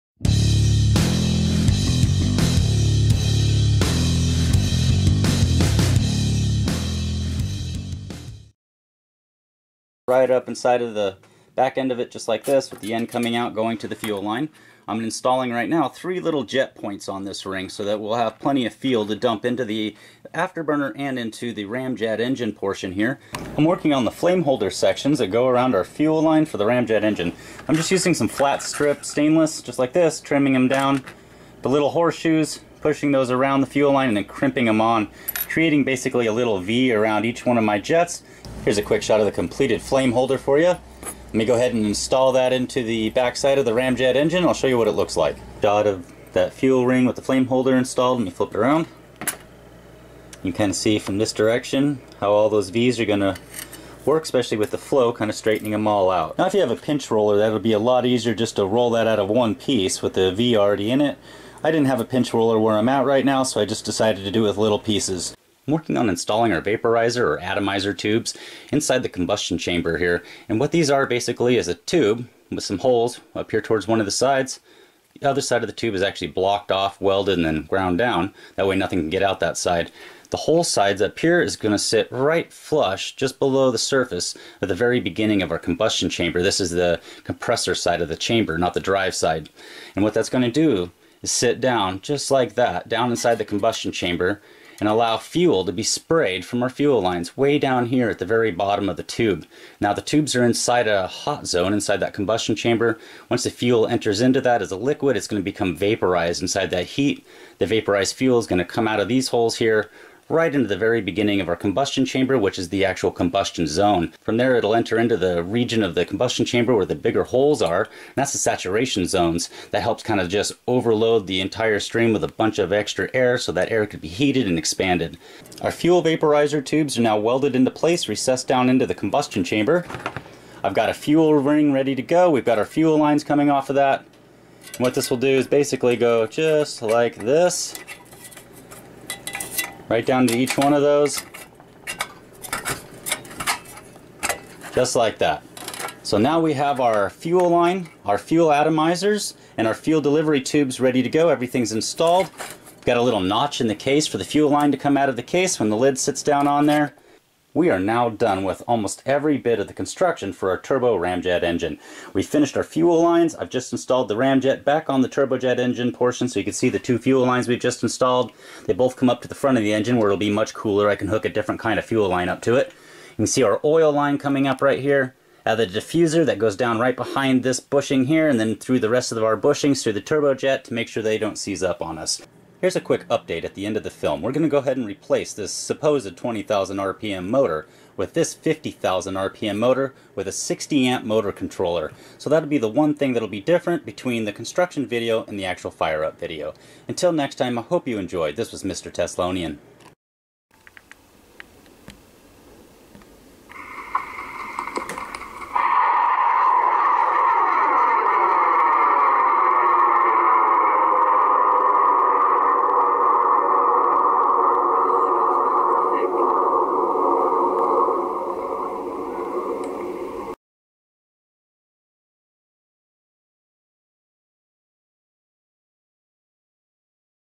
Build a SR-71 Black Bird's TURBORAMJET engine, Part 4
How to build your own Turboramjet engine from the ground up, this is the same engine that is found in the SR-71 Black Bird. This engine uses a two stage compressor system similar to the Rolls Royce Dart engine to power the main combustion chamber, the afterburner and the ramjet sections of the engine. The full length film is not in HD so I am providing these HD versions of the film for builders that may want more detail in the film.